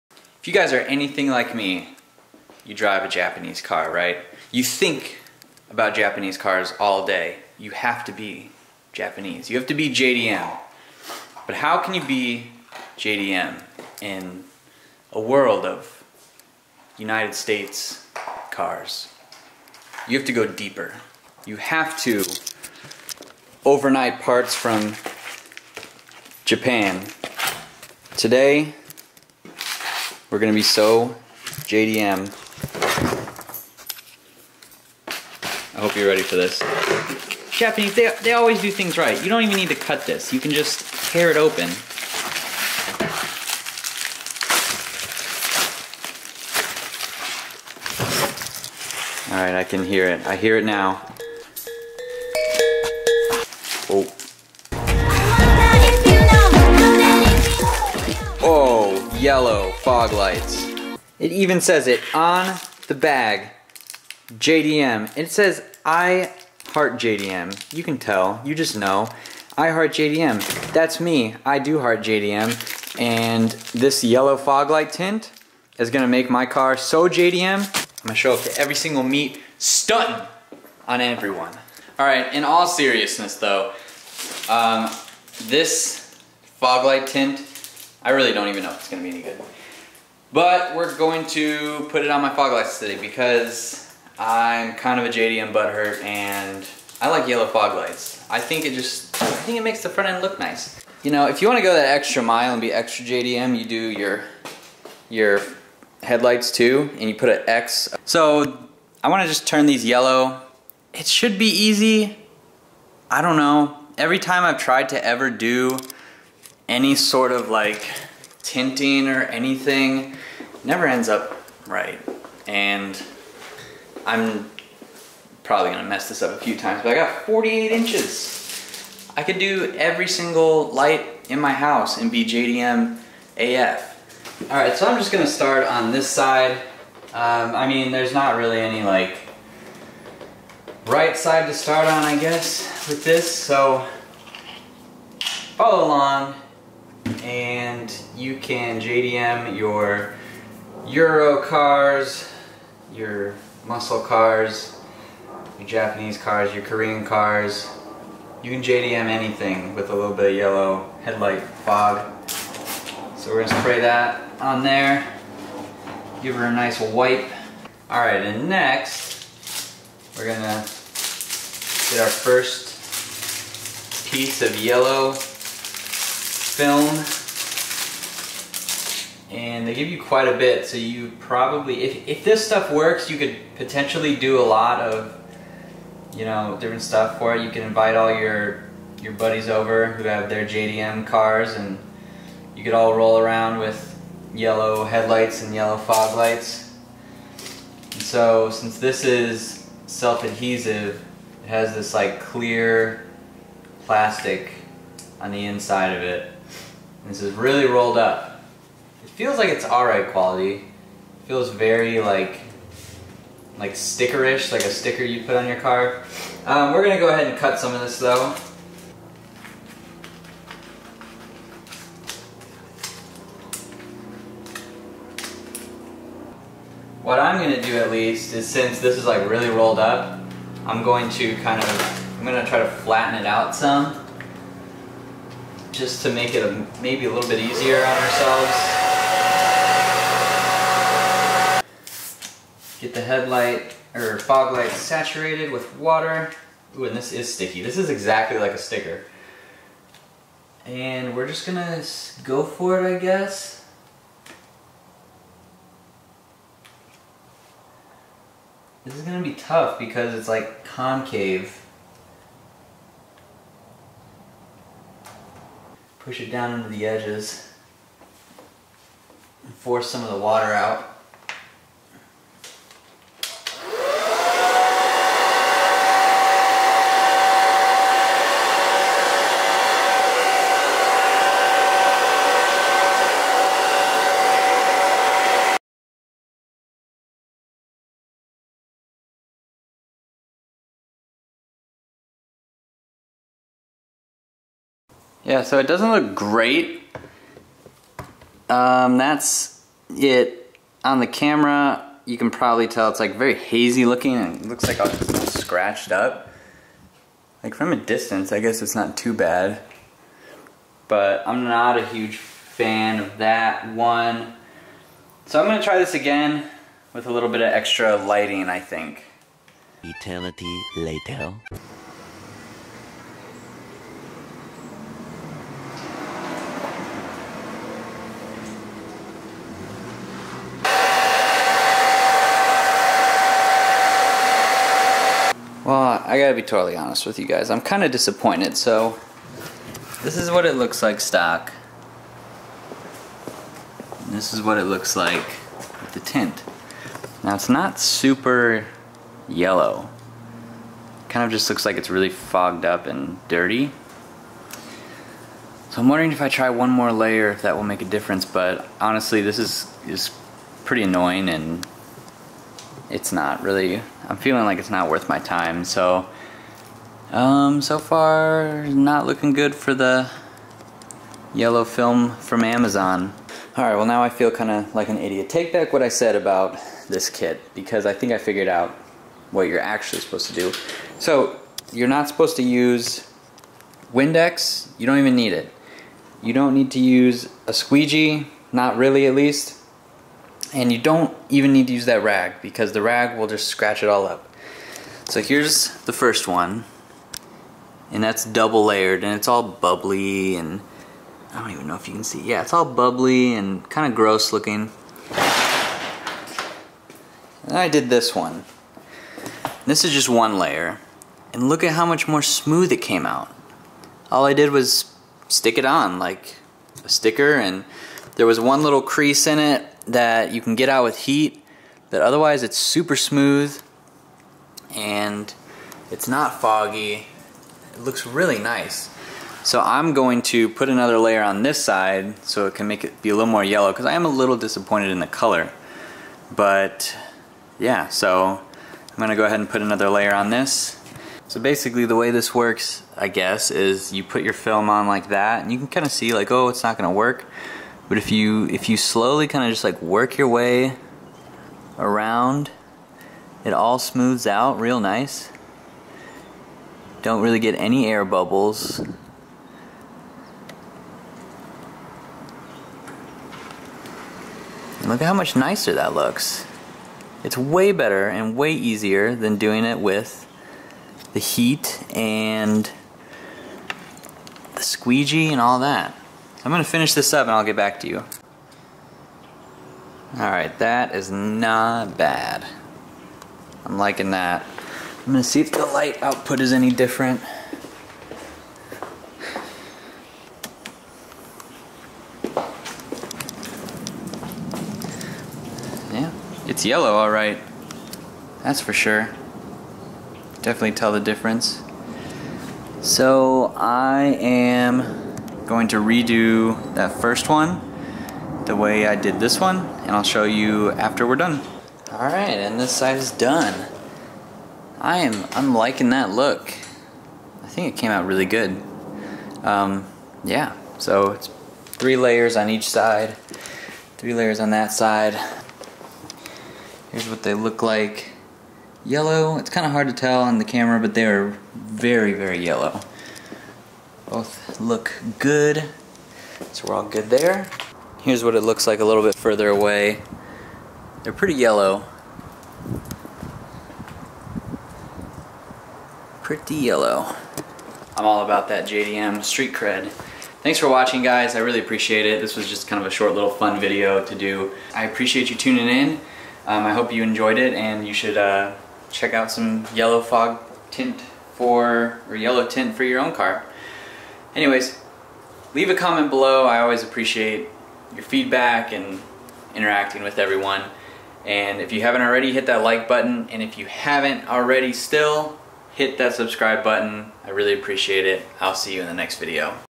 How to JDM Your JDM - Yellow Fog Light Tint!
Street cred instantly rises! Fun video on installing yellow fog light tint.

Gamertag ► ColoredinLight